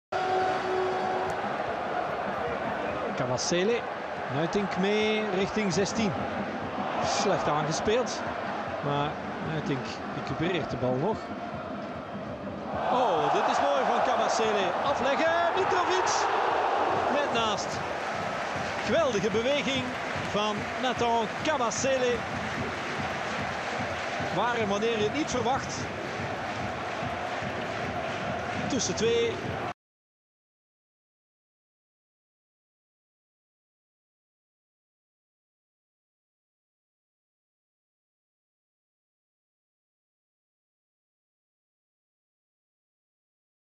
Nathan Kabasele amazing trick
An amazing trick of youngster Nathan Kabasele. He is the nephew from Dieumerci Mbokani. Just sit & enjoy this wonderful trick. Just stunning. This kid is a genius.